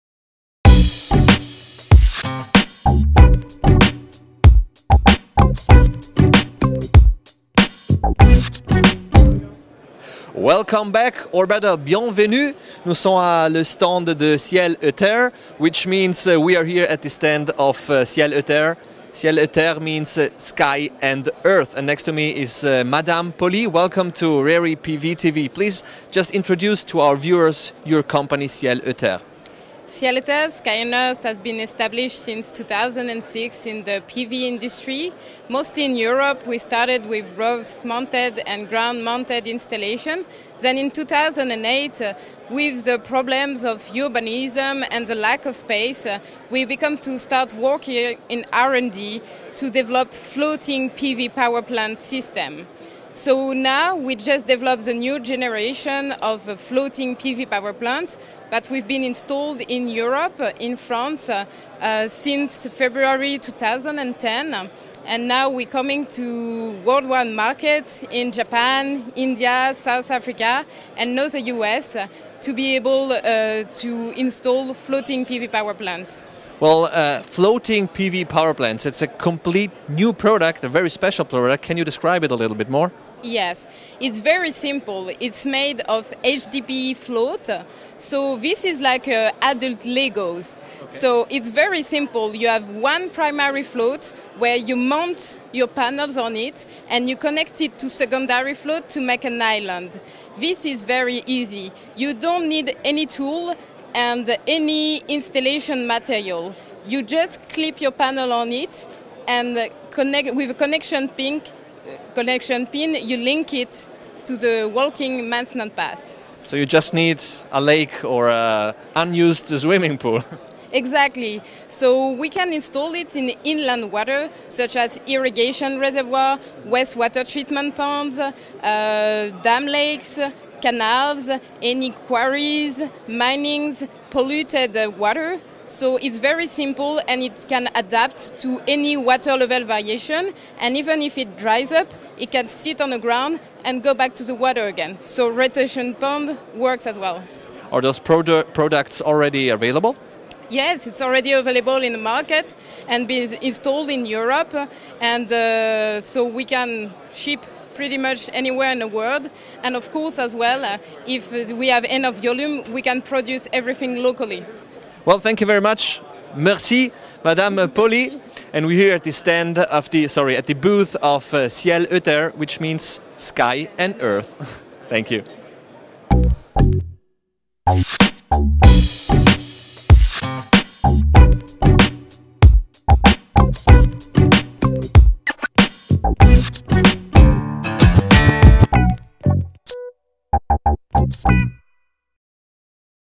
RERi PV.tv Intersolar North America 2012 - Interview Ciel et Terre
RERi PV.tv presents Eva Pauly, Solar Systems Business Development Manager of Ciel et Terre, and their floating solar power plants.